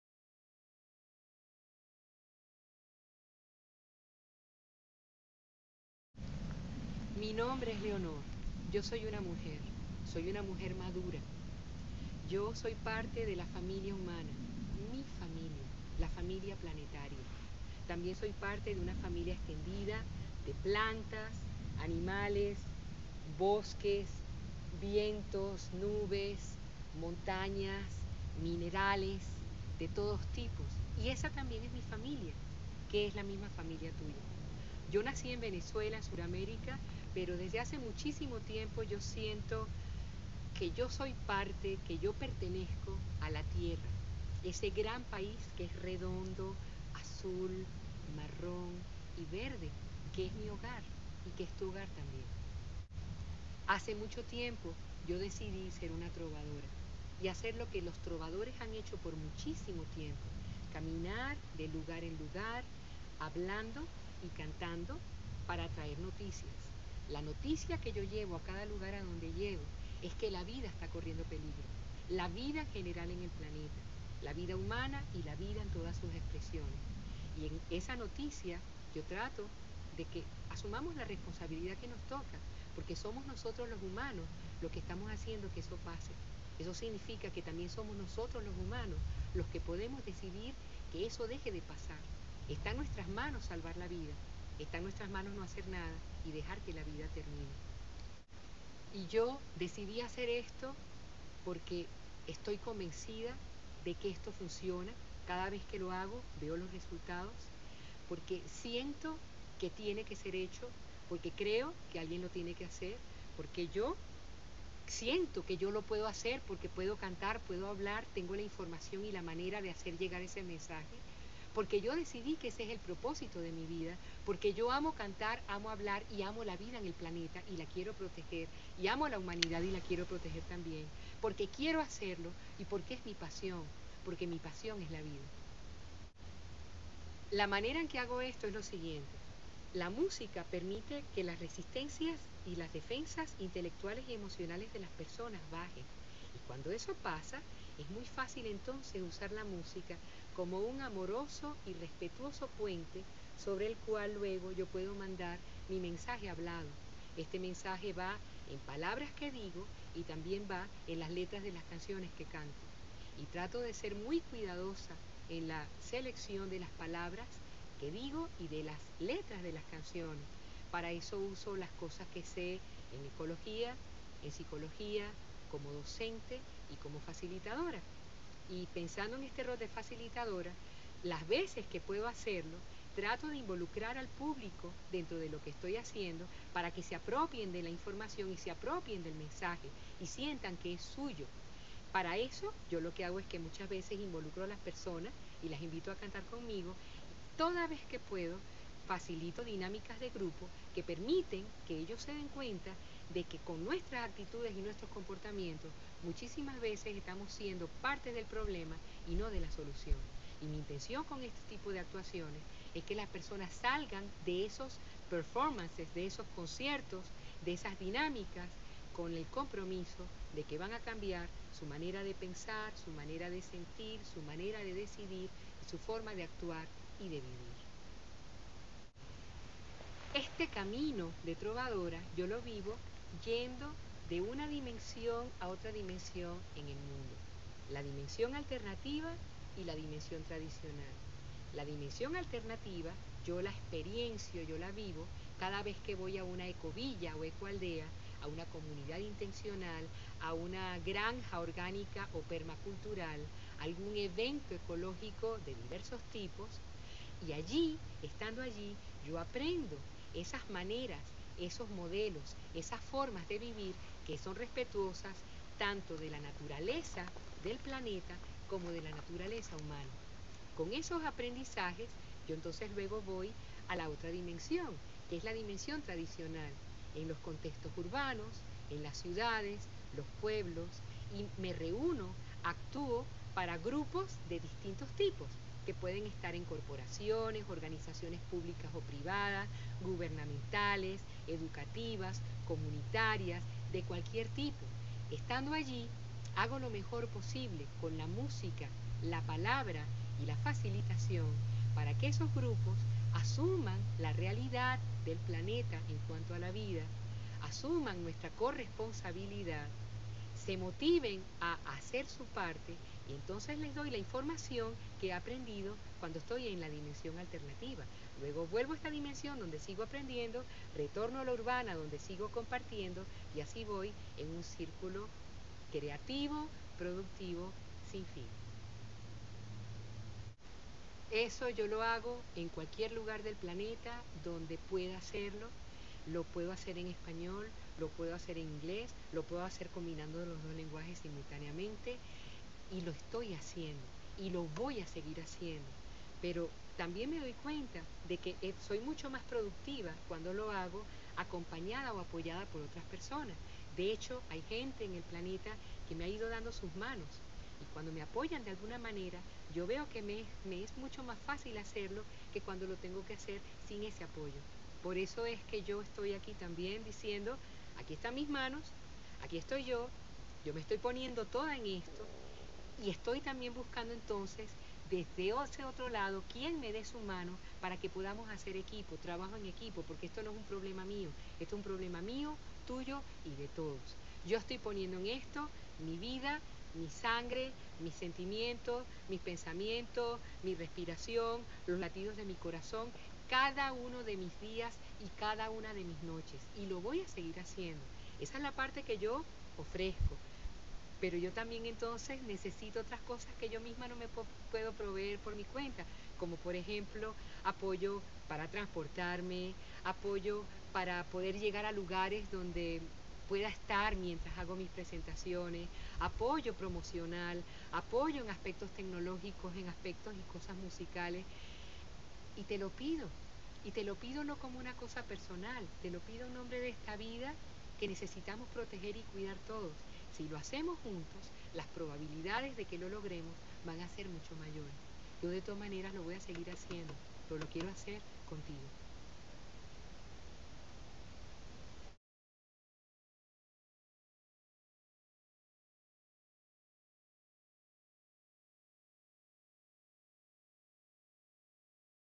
¡Hola¡ yo soy Leonor Fuguet, Trovadora Ecológica
Esta es una presentación de mi trabajo musical como Trovadora Ecológica: lo que hago, como lo hago, por qué lo hago y lo que necesito para seguir haciéndolo.

Para escuchar mi música, haz clic en mis otros videos.

También hay versiones de mi presentación y música en inglés